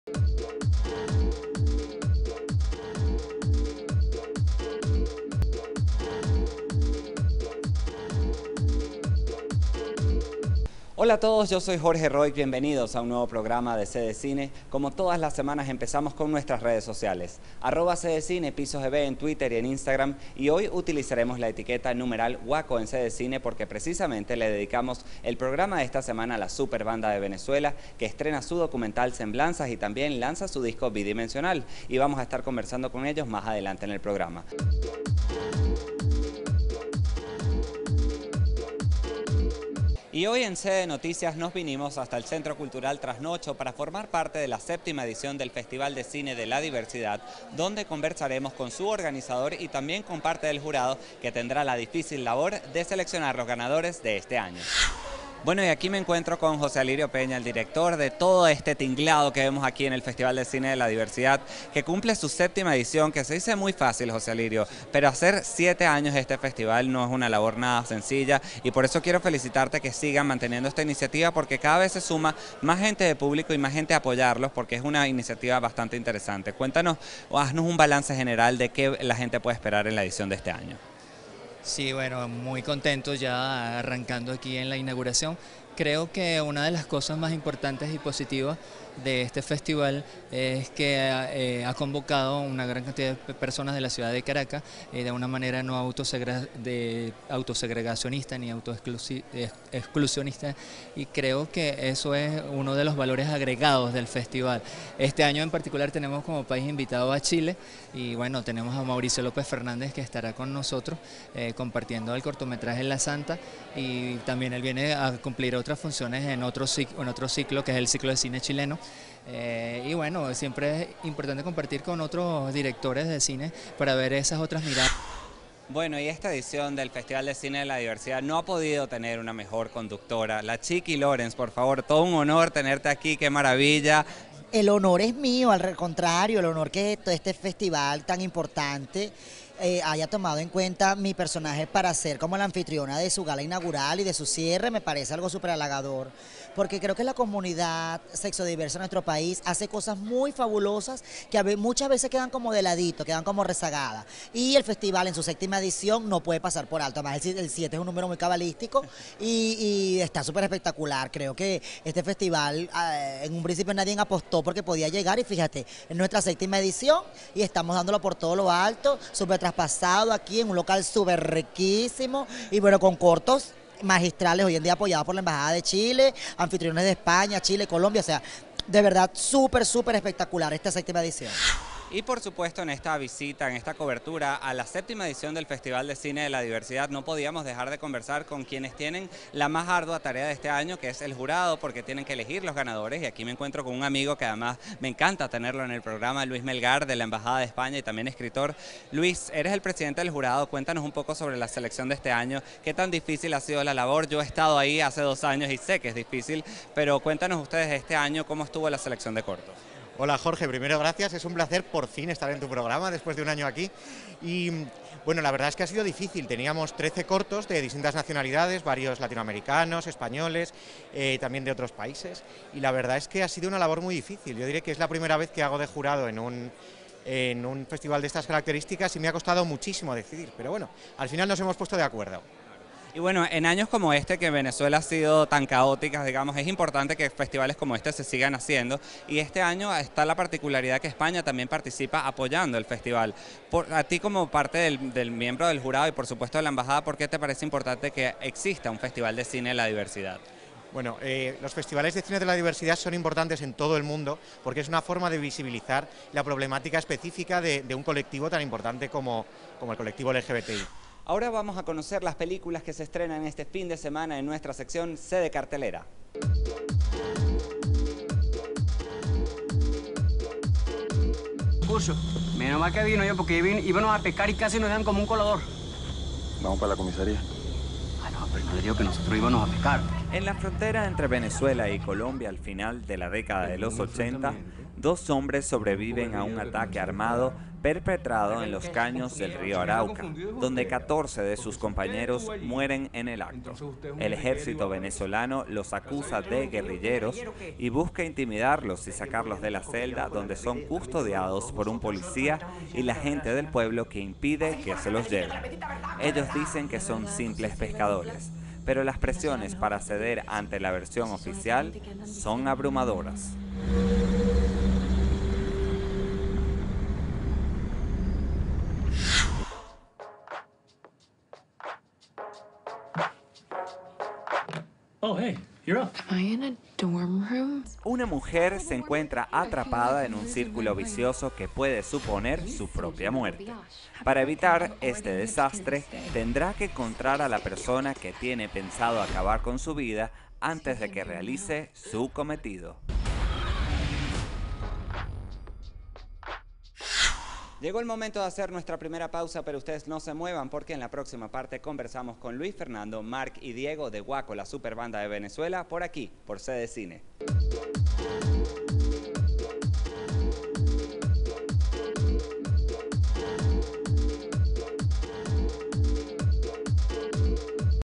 C de Cine: Festival de Cine de la Diversidad y estreno de documental de Guaco (Parte 1/3)
CdeCine estuvo presente en la séptima edición del Festival de Cine de la Diversidad dónde pudimos conversar con su director, Jose Alirio Peña, con la conductor del eventro, La Chiki Lorenz y con el presidente del jurado, el diplomático y escritor español Luis Melgar.

Además celebramos junto a la súper banda de Venezuela, Guaco, el estreno de su película documental, Semblanza dirigida por Alberto Arvelo. Luis Fernando, Diego y Mark nos cuentan todos los detalles sobre la grabación de esta esperada película venezolana que ya se encuentra en cartelera.